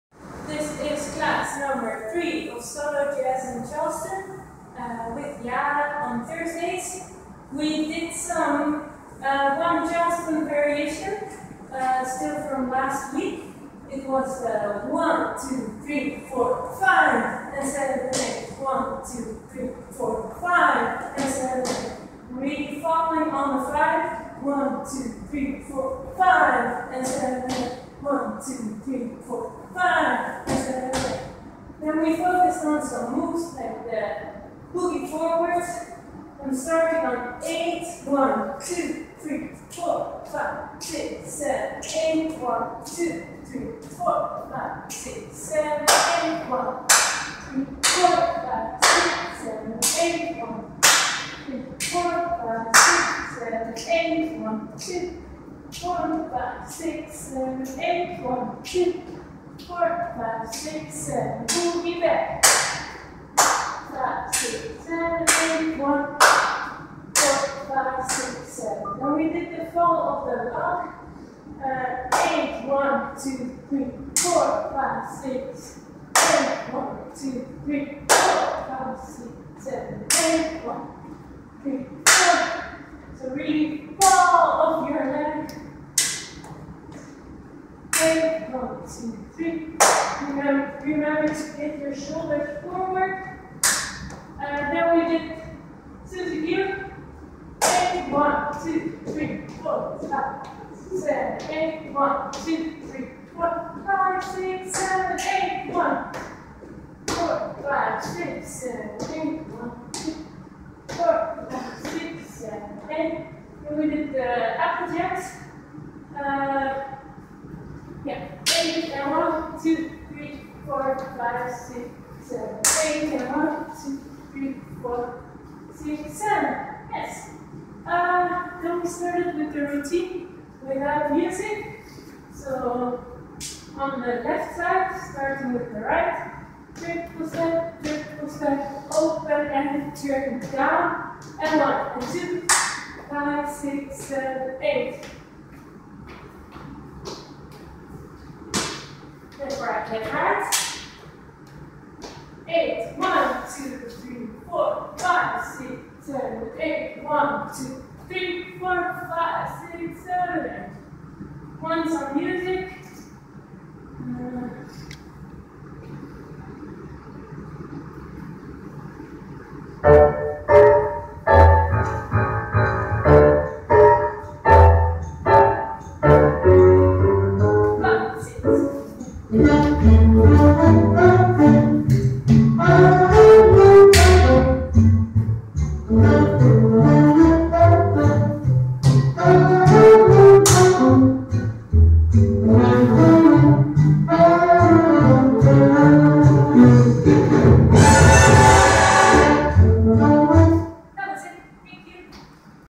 Solo jazz/charleston Yara january 22nd, 3rd class
Solo moves (1. Charleston variation 2. Boogie forward and Boogie backward, fall of the log,
Suzy q, Apple jacks) + first part of routine.